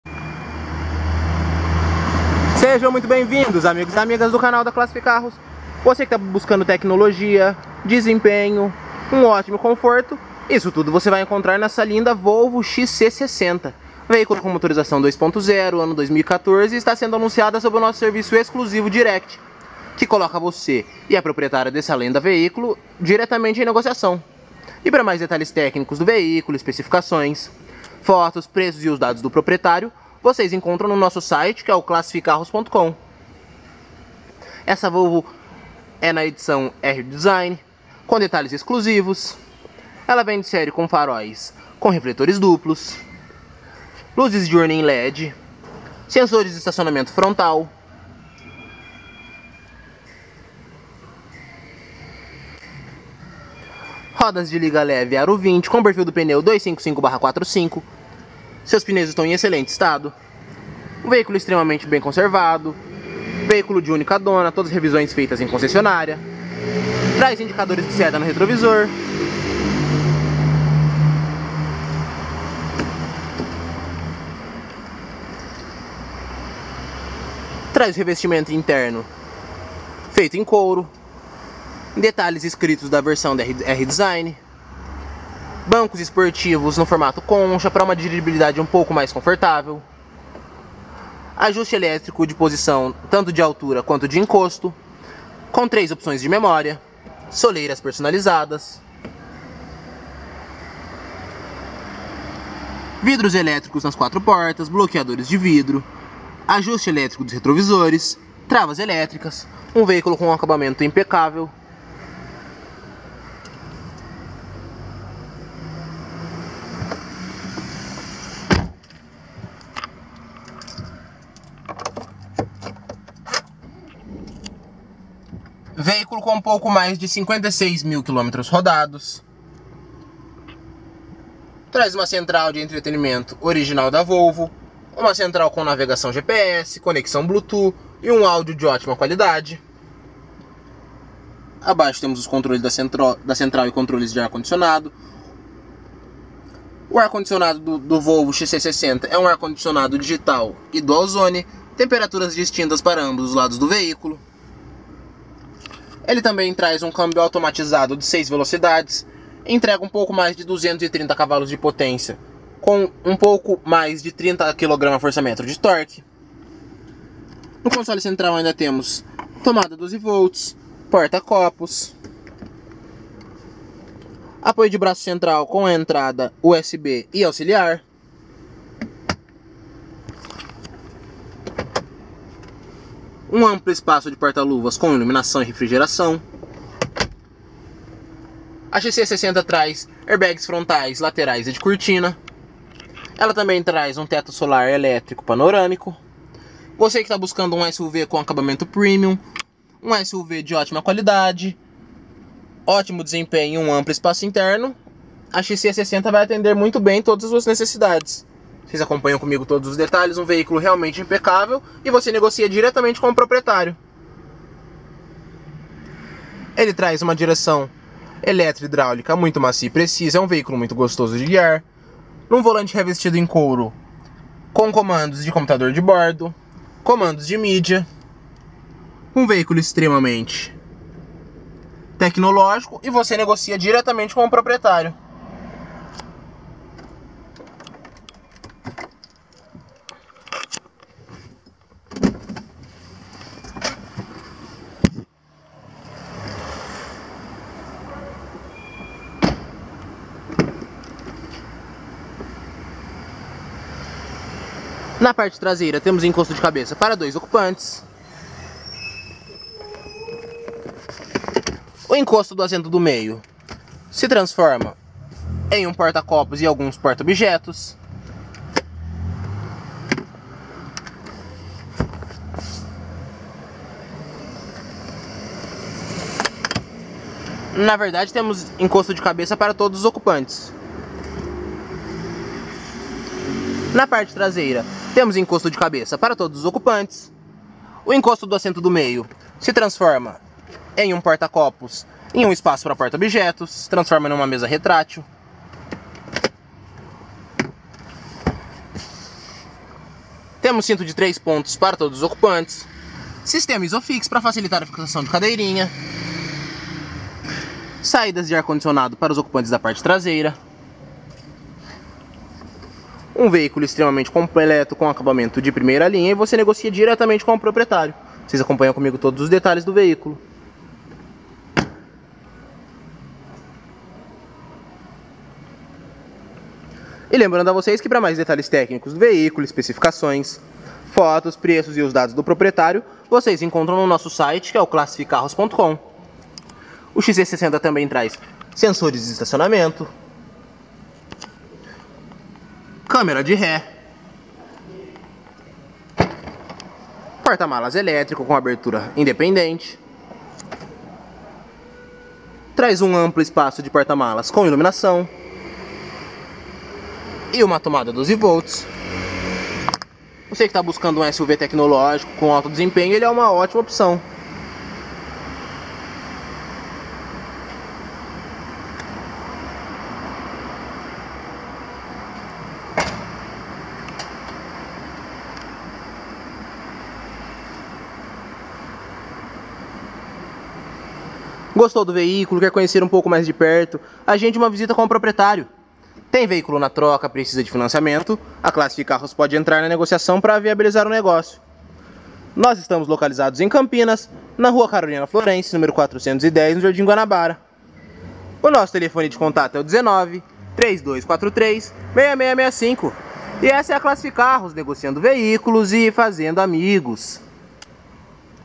Volvo XC 60 2.0 T5 R Design Turbo (Gasolina) Automático 2014
NOVÍSSIMO (ÚNICA DONA). MANUAL E CHAVE RESERVA.  REVISÕES FEITAS EM CONCESSIONÁRIA. O XC60 É O SUV PARA QUEM BUSCA TECNOLOGIA, CONFORTO E DESEMPENHO. VERSÃO R DESIGN CONTA C/ DETALHES EXCLUSIVOS, MOTOR 2.0 TURBO (240 cv), TETO SOLAR E CHAVE PRESENCIAL.